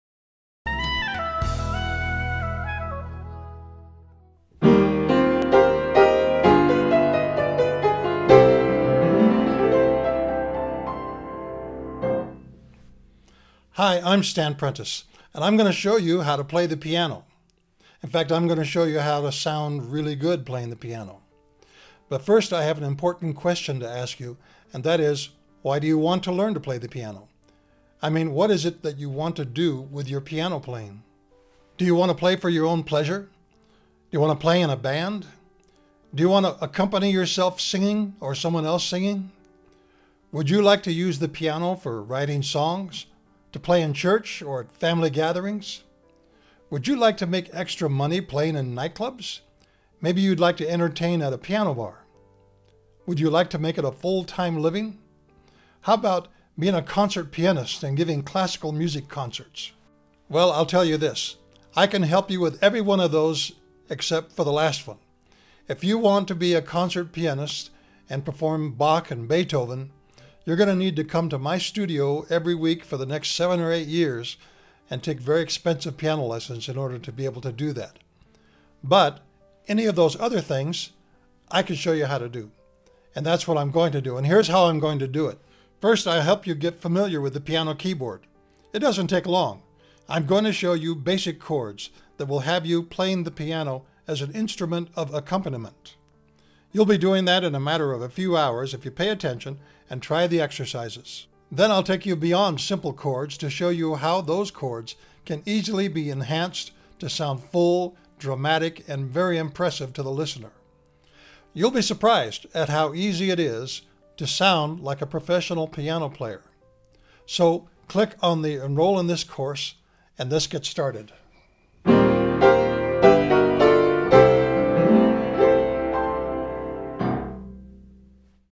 Learn to Play Piano As The Pros Do
Professional piano players who play Popular Music do NOT read music in the traditional way. They use a short cut that is based upon CHORDS. Here is an online course that will teach you that system, and show you how to play great popular songs without years of tedious music lessons. It's called 'Chord-Based Piano.'